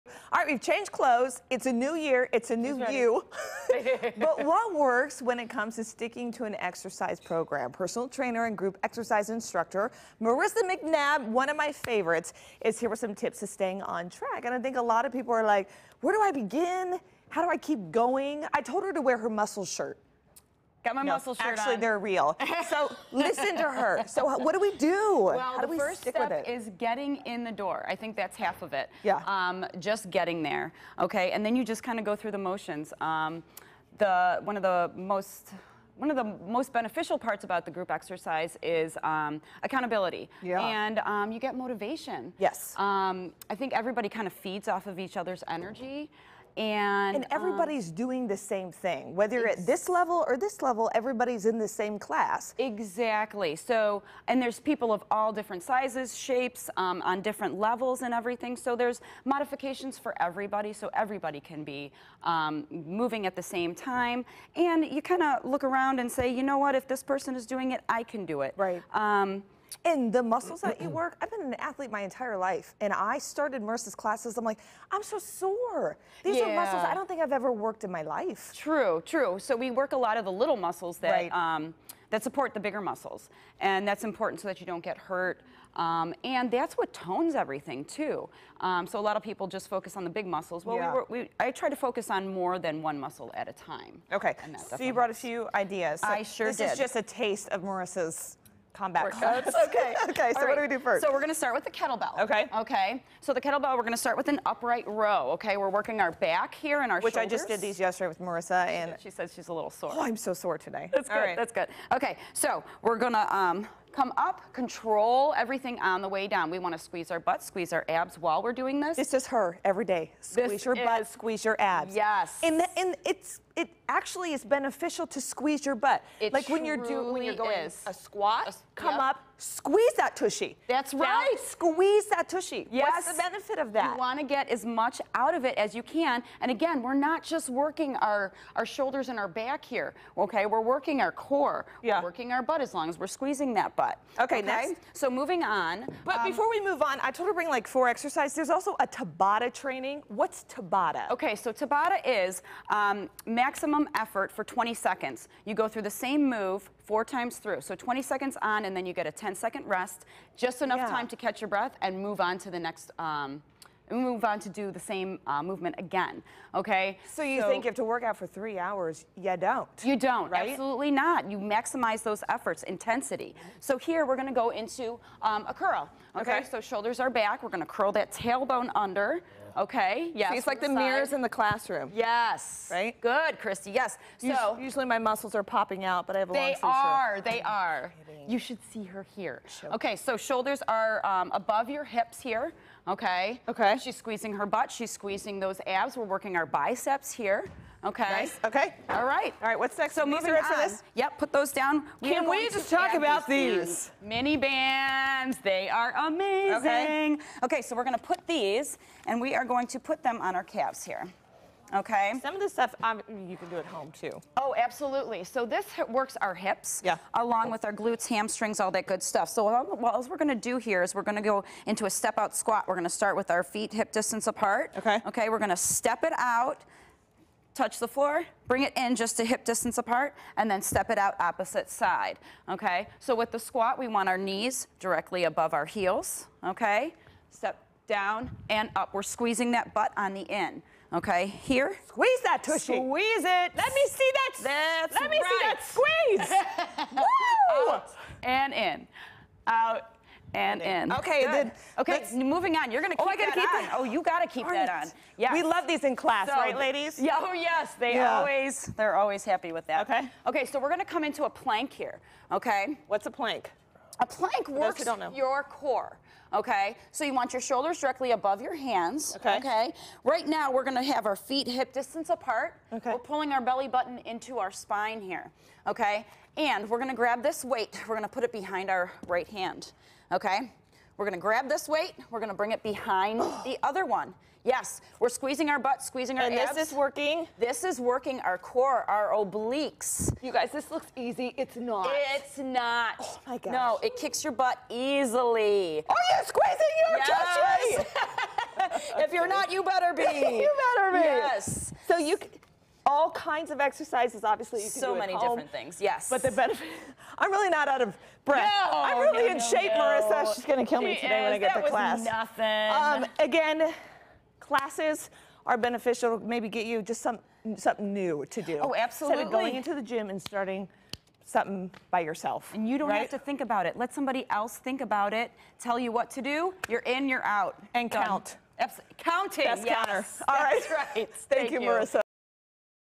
Kristi hits the kettle bells and learns more about the benefits of group exercise
Group exercise class might be more helpful for some people to help them stay on track with their fitness plans. Marissa McNabb is a personal trainer and group exercise instructor and she explained to Fox 8's Kristi Capel the benefits of a group exercise class. Then Kristi got busy giving some of Marissa's favorite exercises a try.